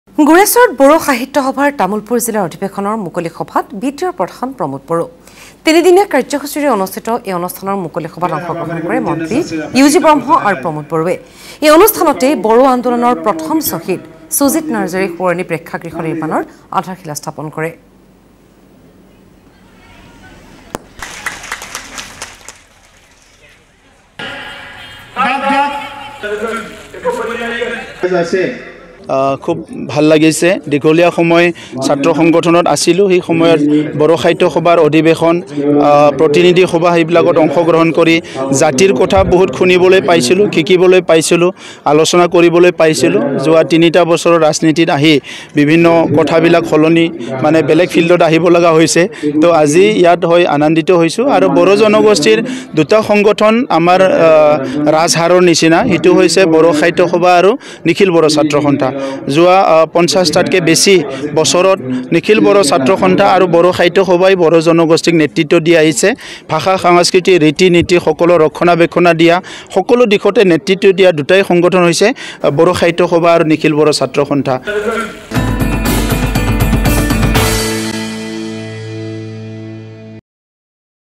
গোৰেশ্বৰত বড়ো সাহিত্য সভাৰ অধিৱেশনৰ মুকলি সভাত BTR প্ৰধান প্ৰমোদ বড়ো
📌গোৰেশ্বৰত বড়ো সাহিত্য সভাৰ তামুলপুৰ জিলাৰ অধিৱেশনৰ মুকলি সভাত BTR প্ৰধান প্ৰমোদ বড়ো। এই অনুষ্ঠানতে বড়ো আন্দোলনৰ প্ৰথম শ্বহীদ সুজিত নাৰ্জাৰী সোঁৱৰণি প্ৰেক্ষাগৃহ নিৰ্মাণৰ আধাৰশিলা স্থাপন কৰে।